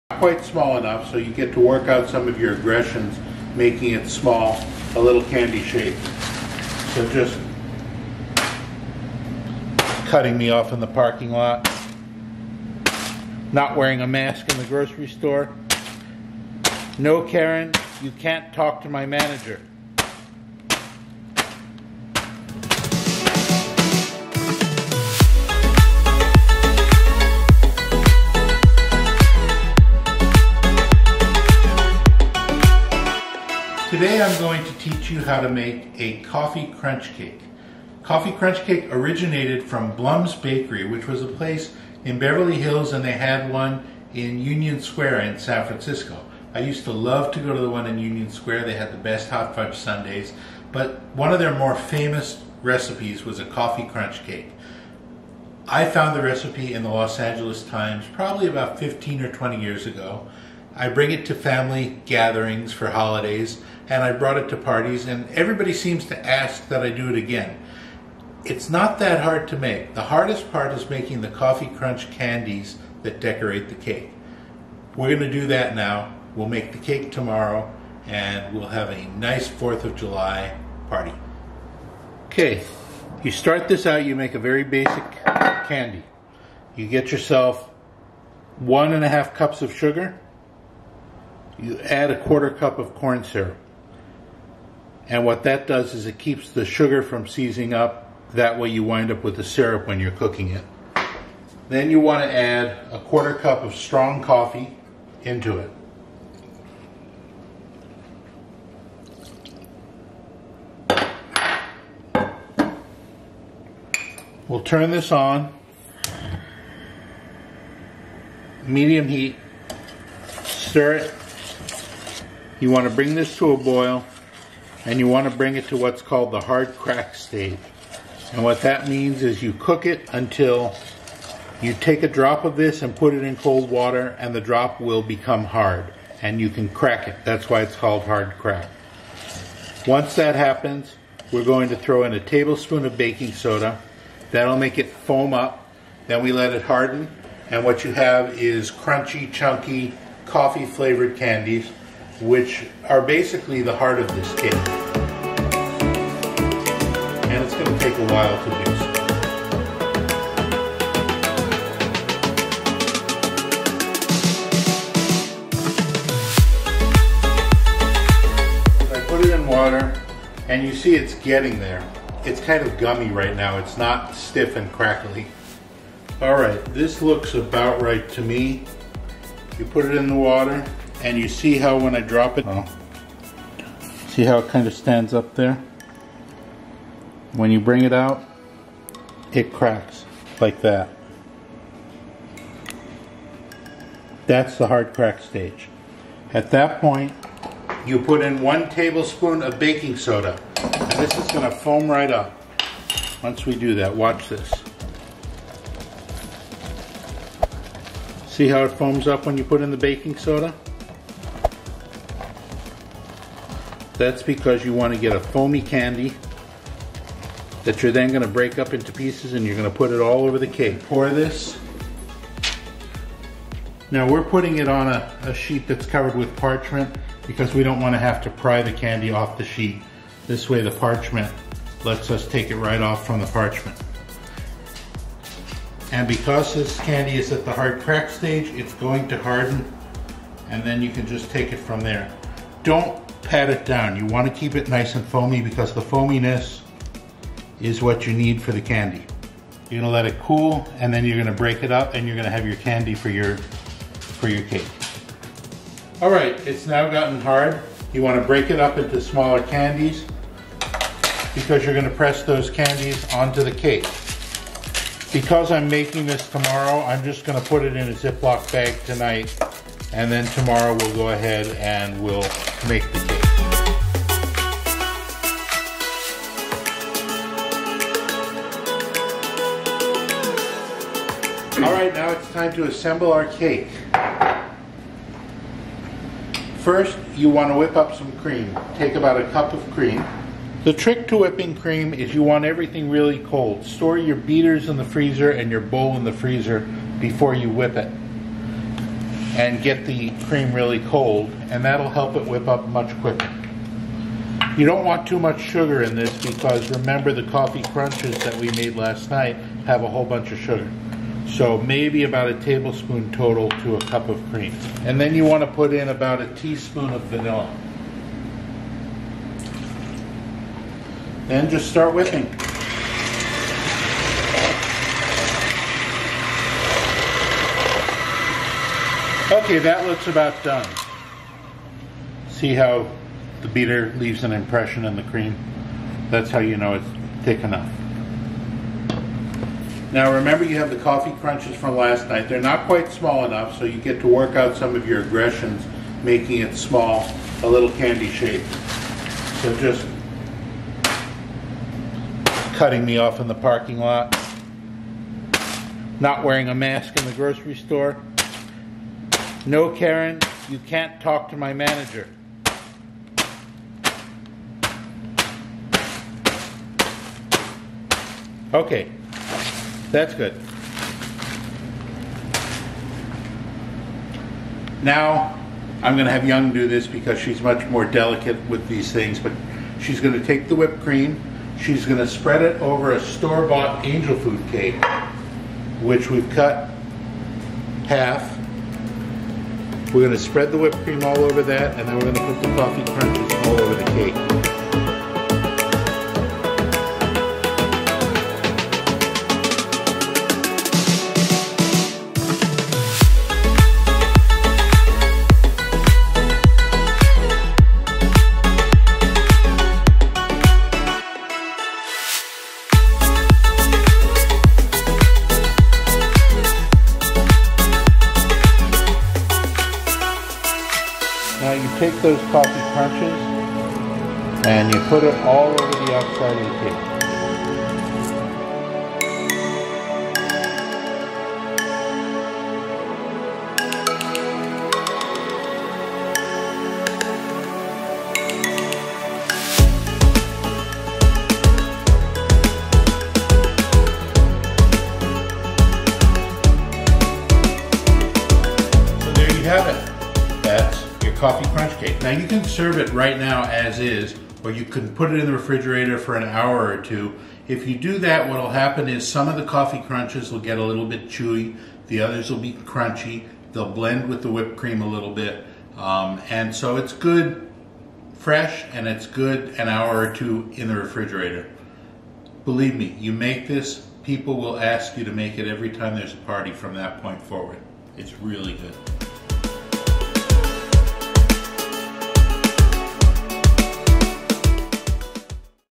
How to Make Blum's Coffee Crunch Cake Recipe Like a Pro
Blum's Coffee Crunch Cake is a legendary dessert that originated from their iconic bakery in San Francisco and Beverly Hills. In this video, we show you how to recreate this delicious cake at home, step-by-step. While it may take some effort, the end result is well worth it. The combination of the light and fluffy cake layers, coffee-infused frosting, and crunchy caramelized coffee crunch is simply irresistible. Impress your friends and family with this amazing dessert and become the talk of the town.